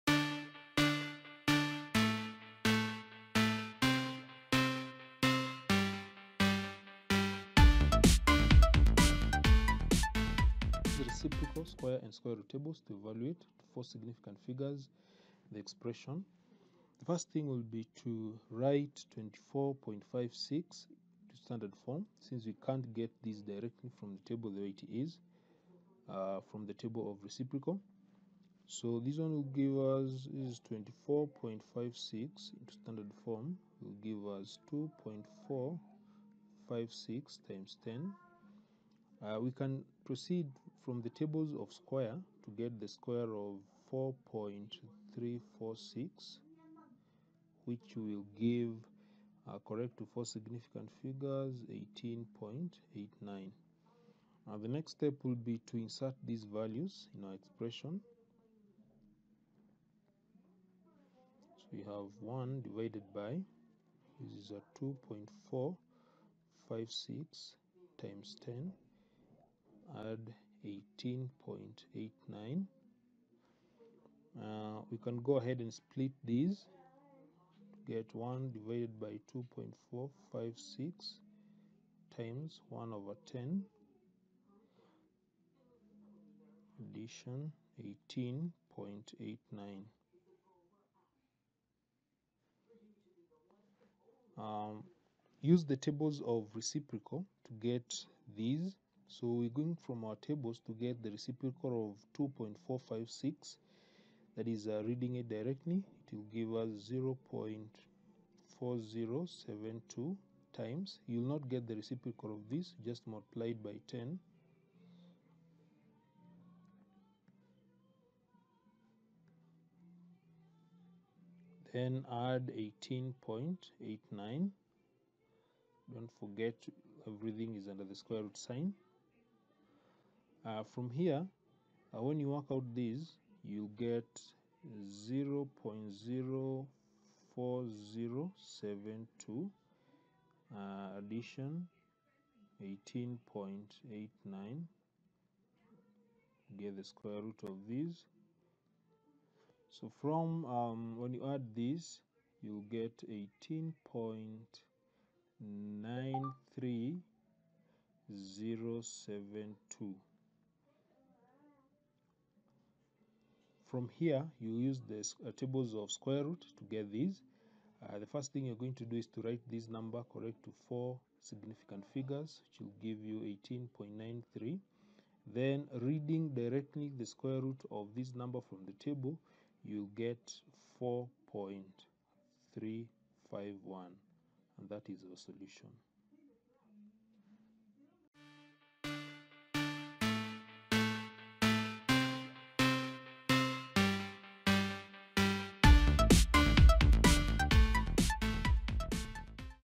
Use of squares,square roots and reciprocal mathematical tables
A Video illustrating using an example on the use of squares,square roots and reciprocal mathematical four figure tables to evaluate problems.